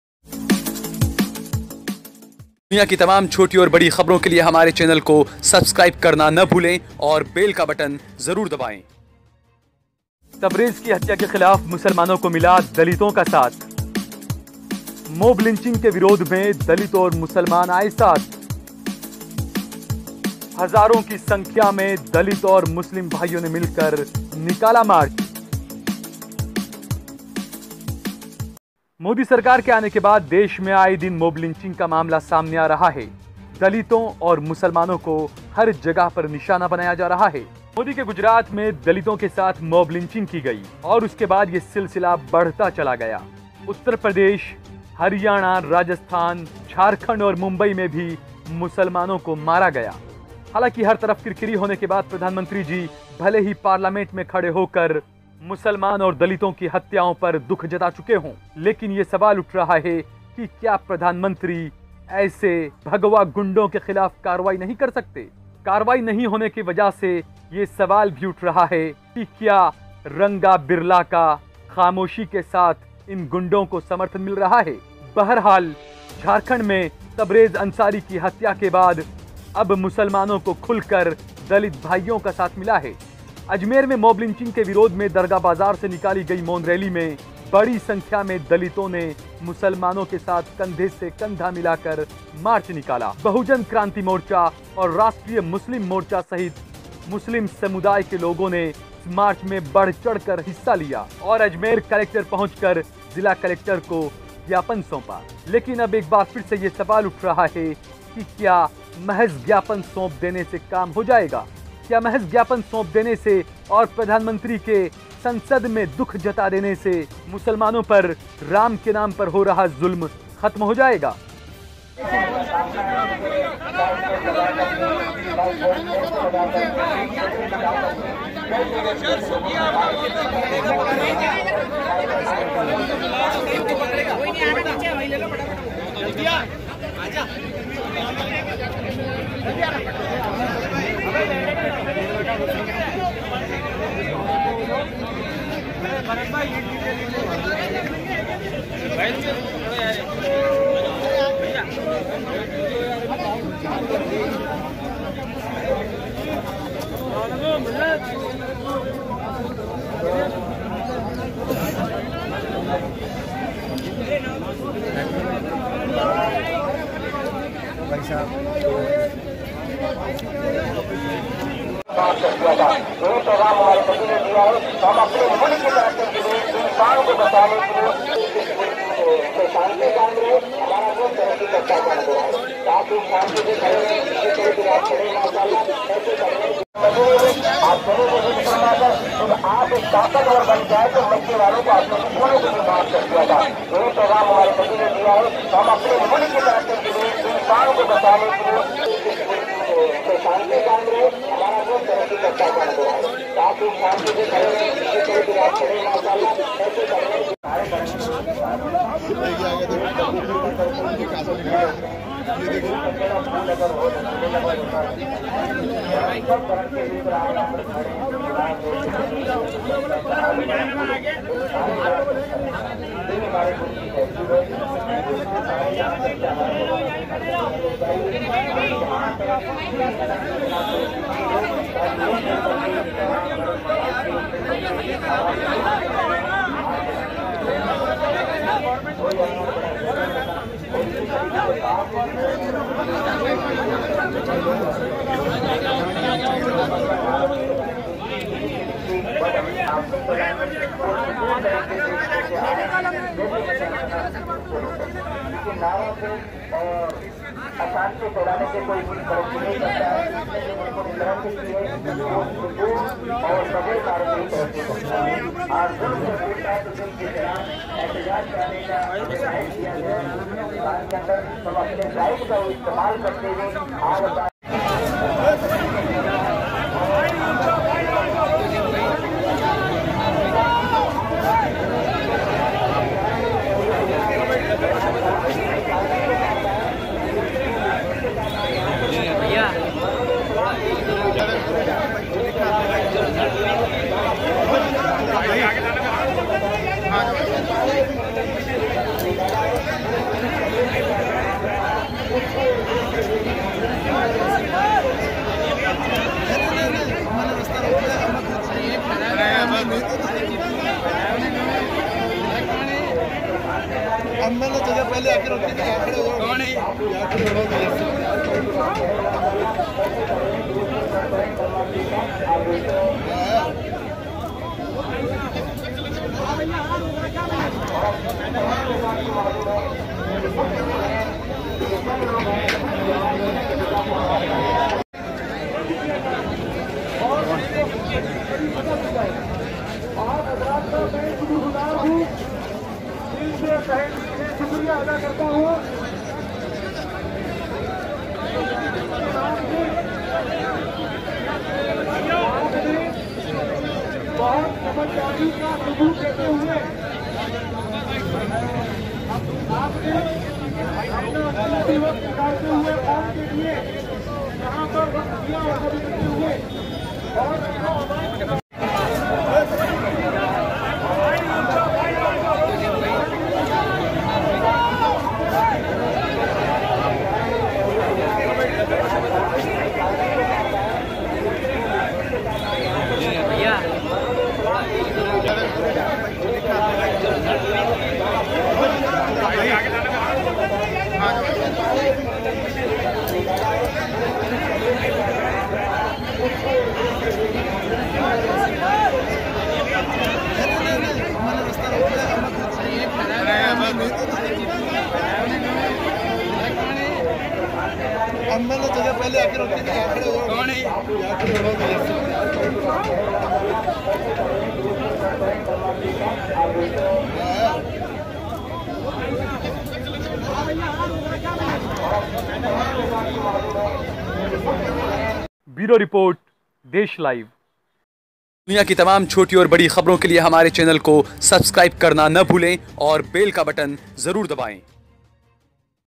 तबरेज़ को न्याय दिलाने के लिए हज़ारों दलित भी उतरे सड़कों पर, मुसलमानों के साथ अजमेर में निकाला मार्च
तबरेज़ को न्याय दिलाने के लिए हज़ारों दलित भी उतरे सड़कों पर, मुसलमानों के साथ मिलकर अजमेर में निकाला शांति मार्च।

Dalit demands justice for Tabrez, Dalit-Muslim protest with muslims in Ajmer.